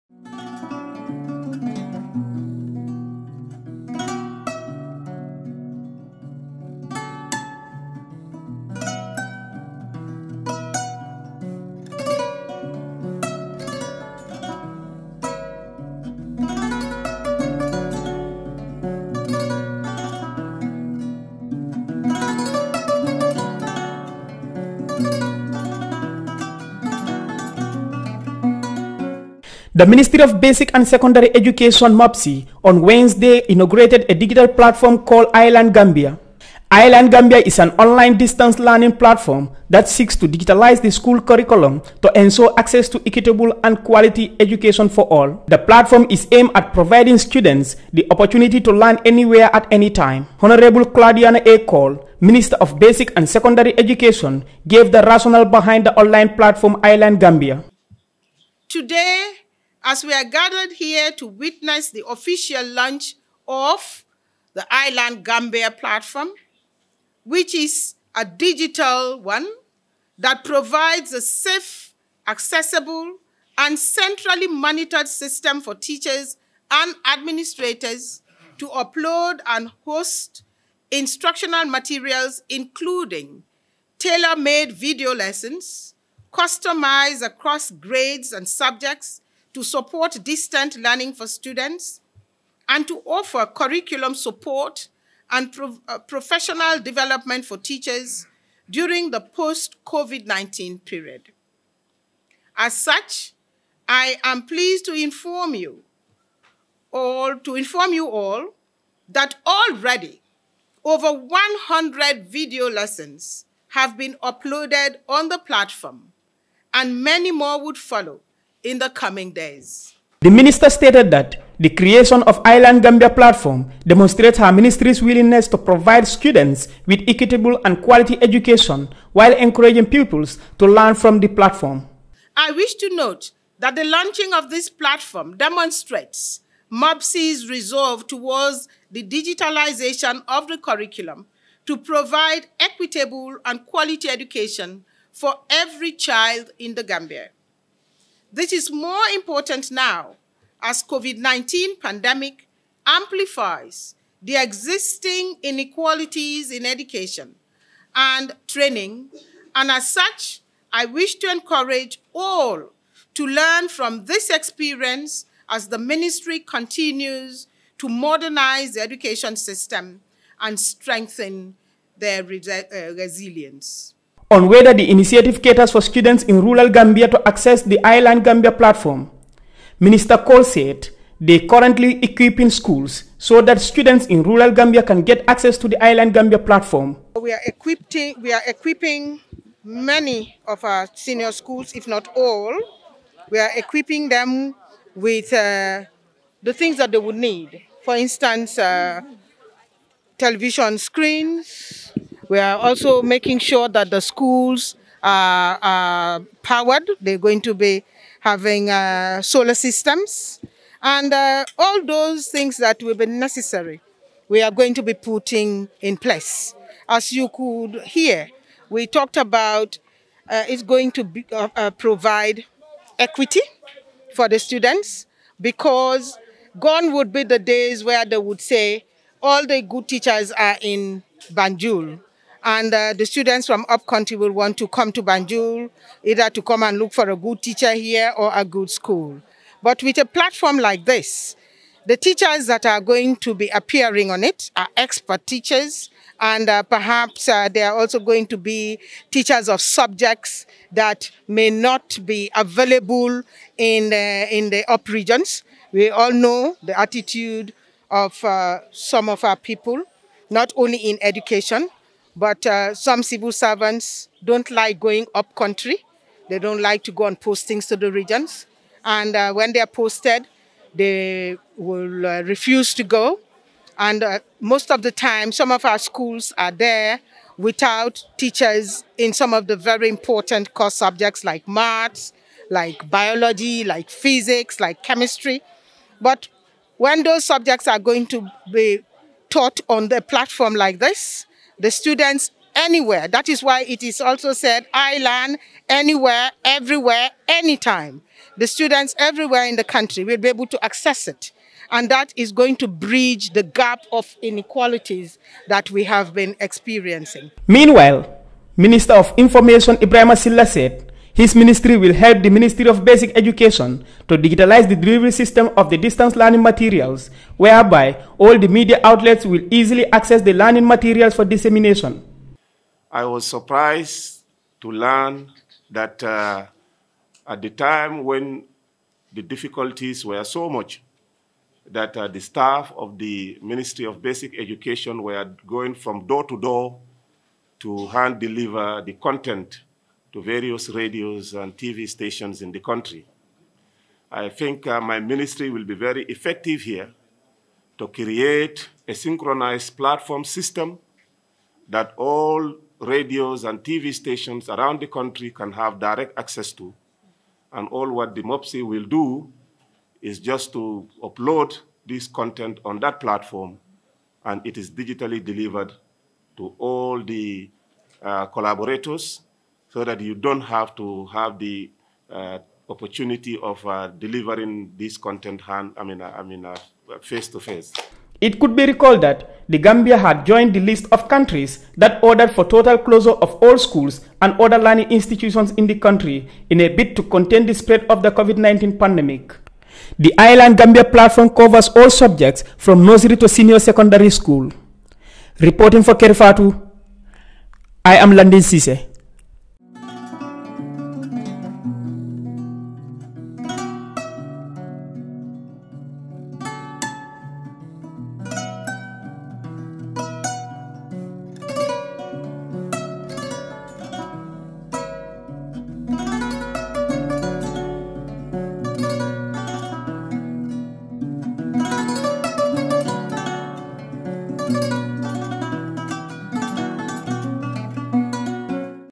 (MoBSE) launched an online learning platform for Nursery to Secondary "ilearnGambia platform"
Ministry of Basic and Secondary Education (MoBSE) launched an online learning platform for students from Nursery to Secondary School called"ilearnGambia platform"